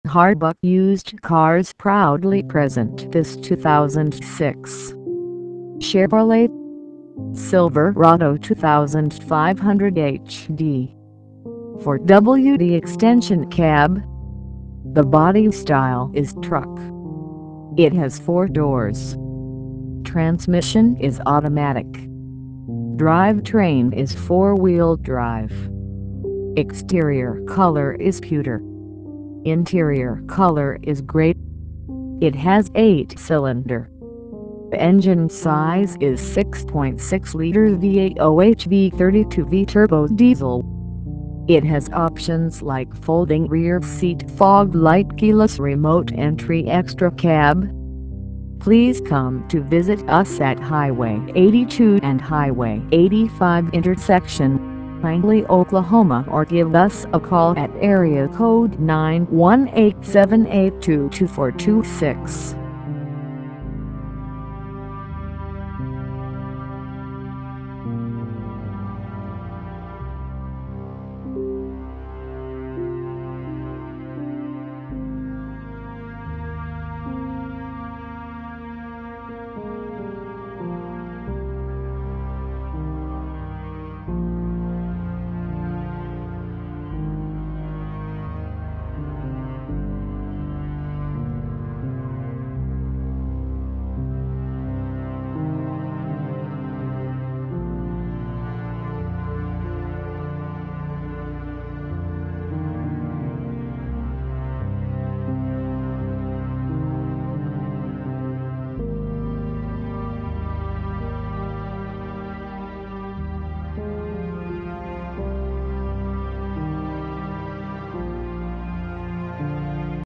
2006 Chevrolet Silverado 2500HD $12500 by Harbuck Used Cars
2006 Chevrolet Silverado 2500HD , 4WD Ext Cab, $12500, 240951 Miles , Harbuck Used Cars, check more inventory from this dealership Hwy 82 and Hwy 85 Intersection Langley Oklahoma , , Automatic, 4 wheel drive, Ext Color: Pewter, Int Color:Gray, Body Style: Truck, Engine Size: 6.6L V8 OHV 32V TURBO DIESEL (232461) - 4/18/2016 4:11:11 PM - 4/6/2016 11:09:04 AM.